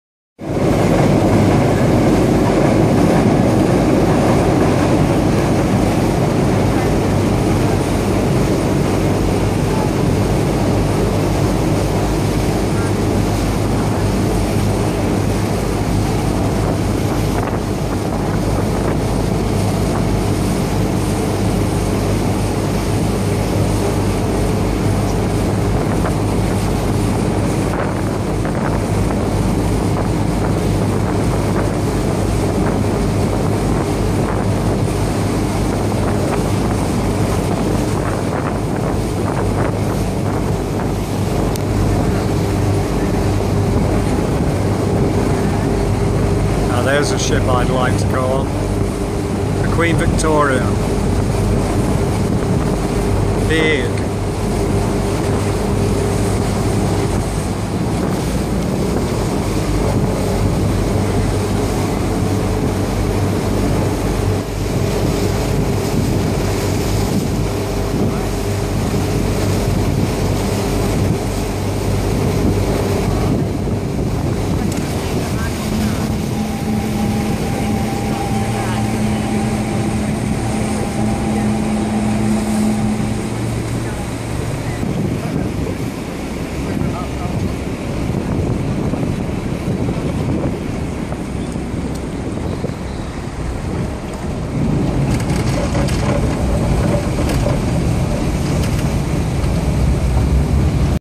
Yesterday; Cunard's Queen Victoria at Circular Quay, Sydney part way on her voyage to Hong Kong.